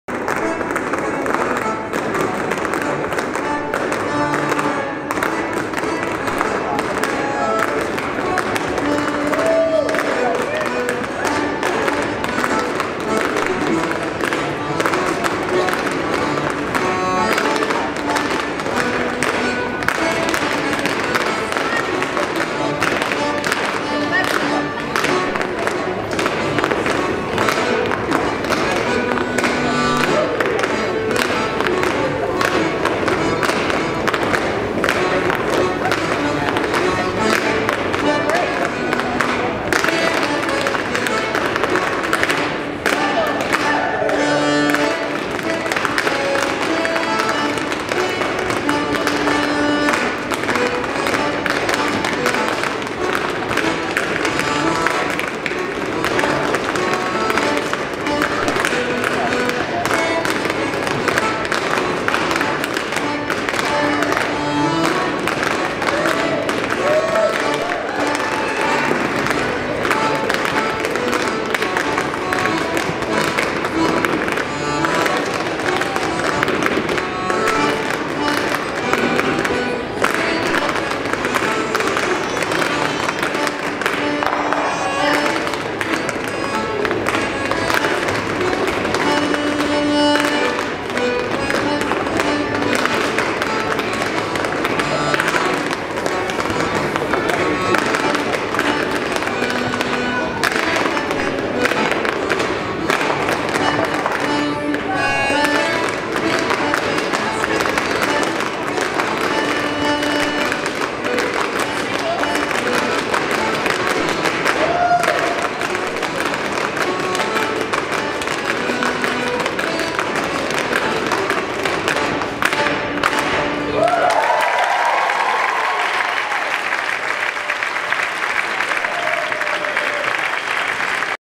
Berea College CCDS - 2015 - Take 6ZF
6ZF - Christmas Country Dance School - Beginning English Clogging, taught and led by Meg Ryan. On the accordion is Karen Axelrod.